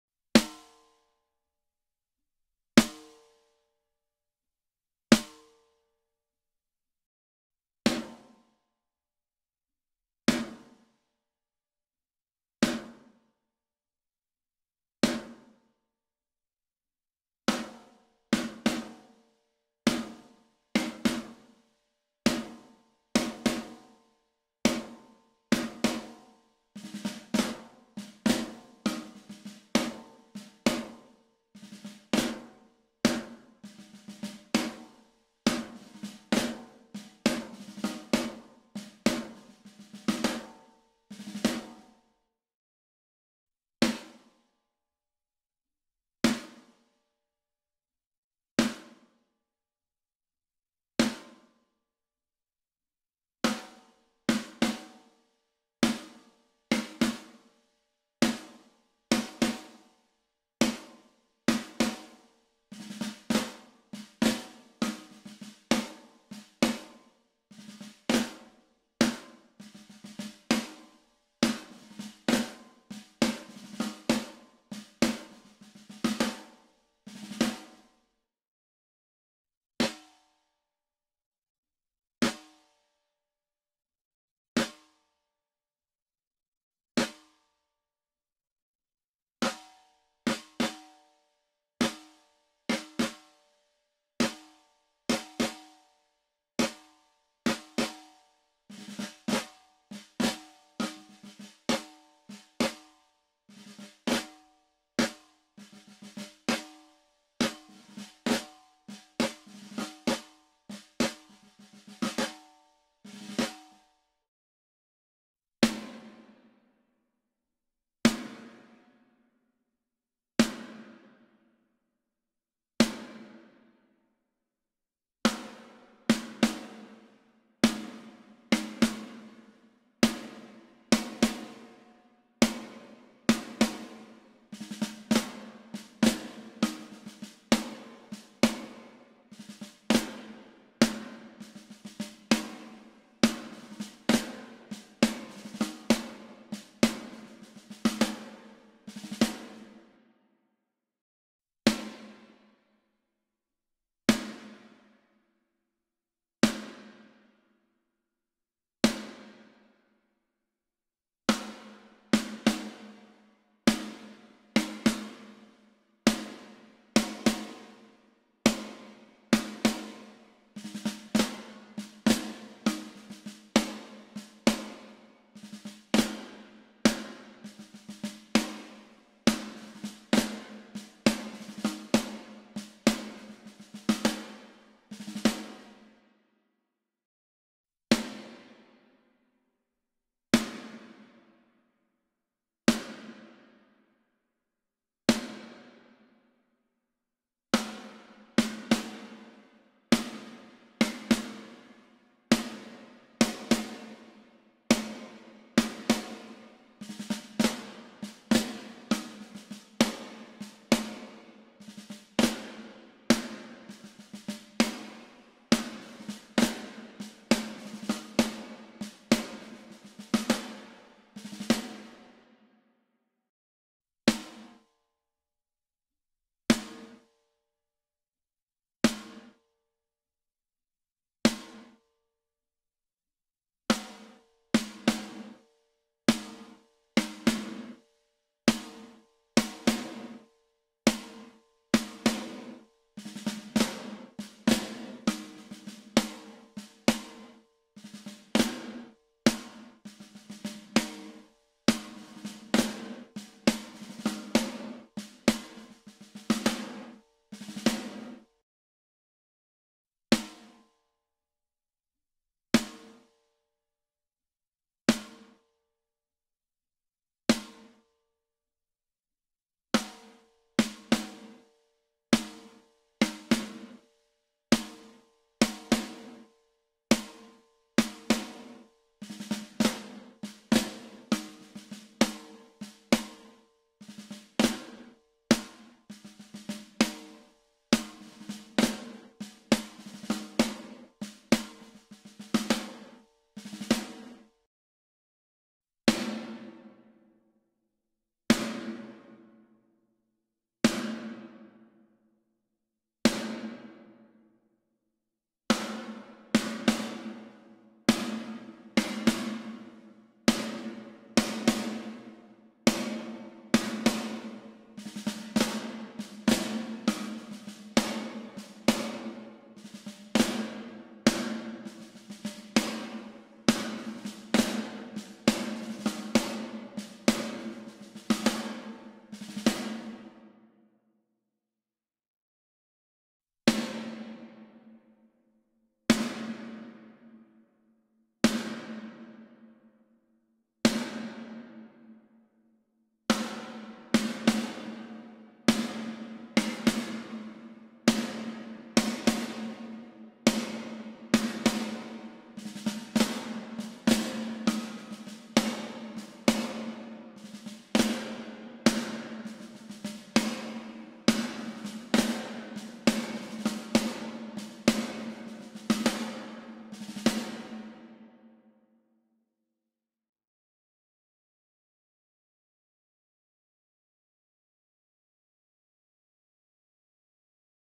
R2 - Ludwig Snare
Example audio with Ludwig Black Beauty Snare.  This drum has a quite noticeable shell resonance.